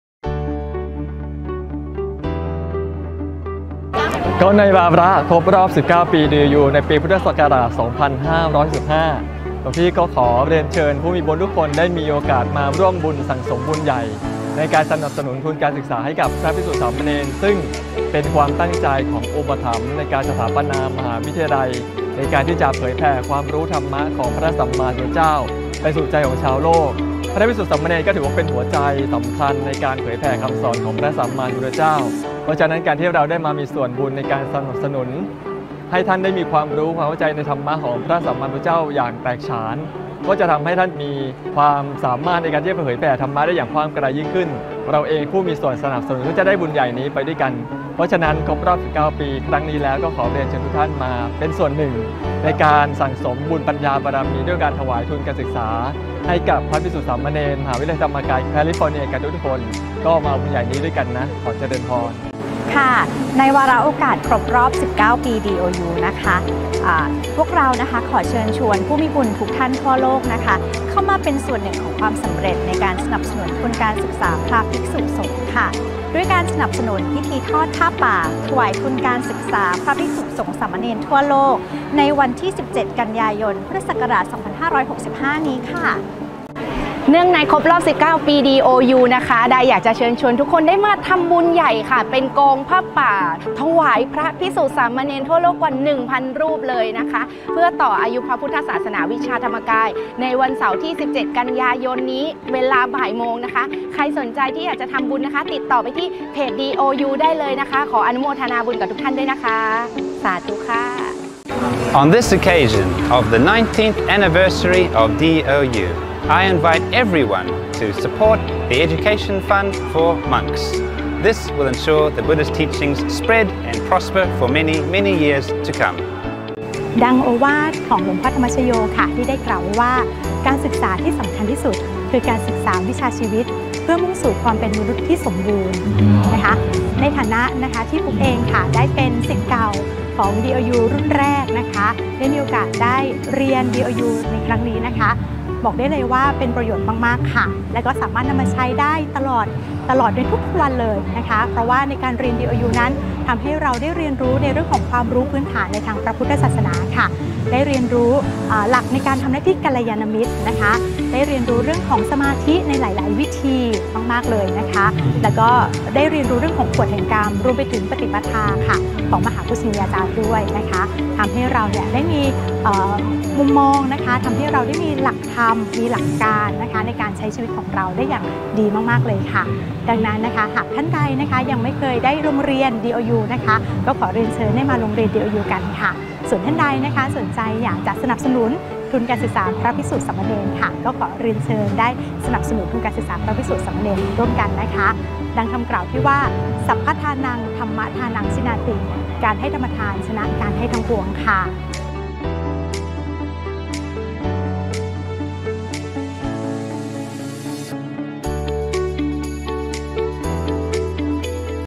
19 ปี DOU ทอดผ้าป้าถวายทุนการศึกษา 17 กันยายน พ.ศ. 2565
🙏เนื่องในวาระครบรอบ19ปี แห่งการสถาปนาDOU มหาวิทยาลัยธรรมกาย แคลิฟอร์เนีย ที่จะจัดขึ้นในวันเสาร์ที่ 17 กันยายน พ.ศ. 2565 เวลา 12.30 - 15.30 น. ณ อาคารอเนกประสงค์(โถงช้าง) วัดพระธรรมกาย
🌎ขอเชิญผู้มีบุญทุกท่านมาร่วมสร้างมหาปัญญาบารมี 📚 ด้วยการถวายกองทุนการศึกษาแด่พระภิกษุสามเณรเรียน DOU ฟรีทั่วโลก ถวายวัดต่างๆทั่วไทย และวัดศูนย์สาขาต่างประเทศ  มาร่วมเป็นส่วนหนึ่งในบุญปลื้มๆแสงสว่างแห่งปัญญากันนะคะ 💰🫧💫
🙏ร่วมบุญได้ที่บัญชี🫧
ธนาคารกรุงไทย
พระวุฒิชัย นิยมวัน
หรือน.ส.เขมิกา วรสันต์โต
ขอกราบอนุโมทนาบุญ สาธุๆ✨✨🙏✨✨